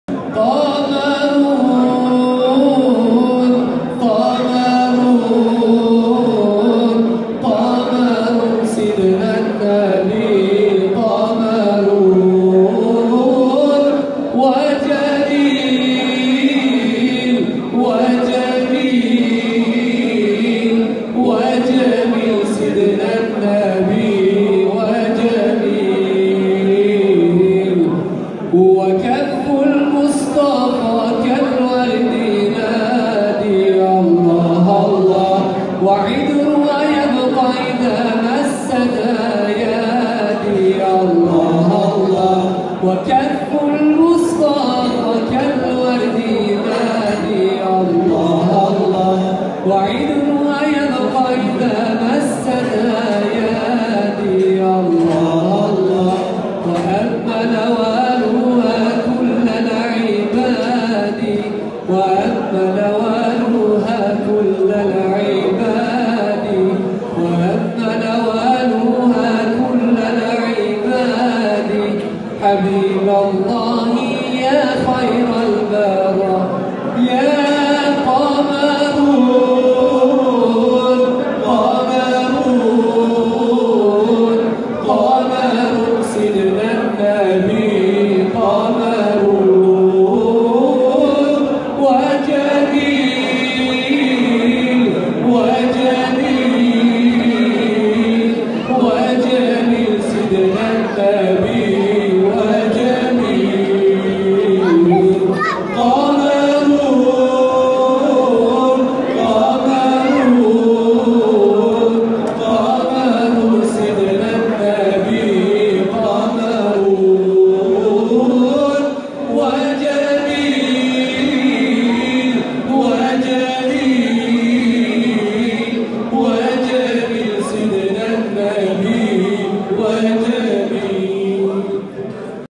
Grupkudüs ilahi grubundan Enes kardeşimizle
Metin Demirtaş, Kopenhag, Danimarka. 18/10 2018.
Grup Kuddüs ilahi ekibi ile düet.